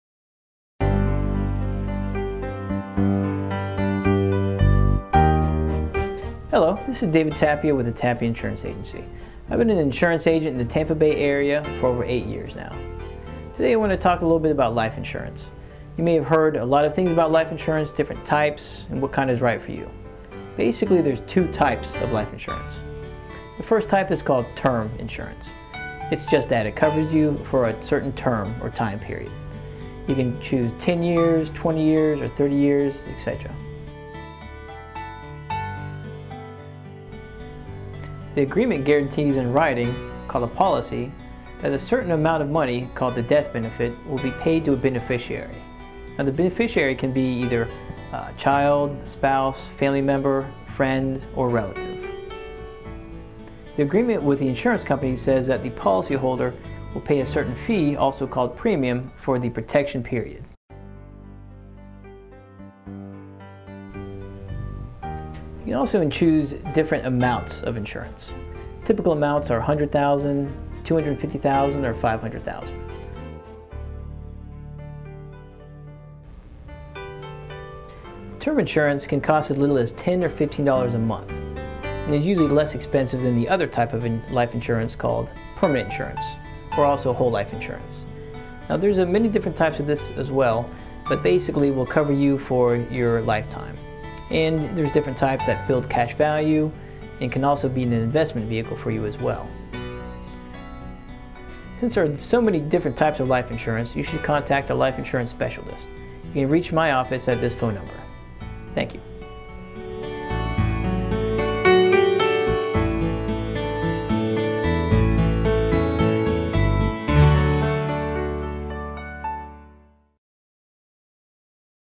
Life Insurance Clearwater - David Tapia's Office
Life insurance coverages explained. When it comes to life insurance decisions, you need to go over your personal and family needs. In order to provide you with a life insurance quote, we need basic information like age, coverage desired, and other important factors.